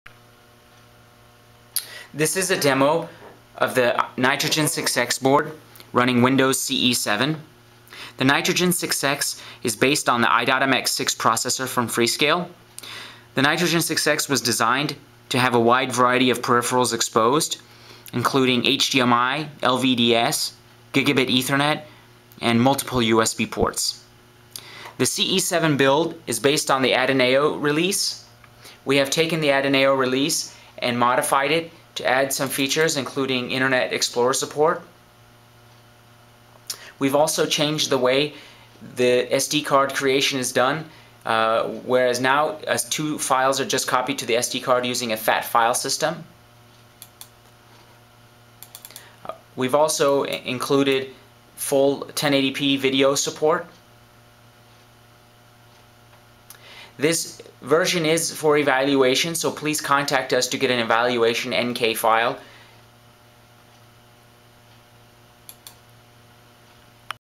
Nitrogen6X SABRE Lite running WindowsCE7
This is demo showing Windows Embedded Compact 7 running on the Nitrogen6X board.  The Nitrogen6X is based on the i.MX6 processor from Freescale.  This demo will also run on the BD-SL-i.MX6/SABRE Lite board as well as the Boundary Devices SOM modules.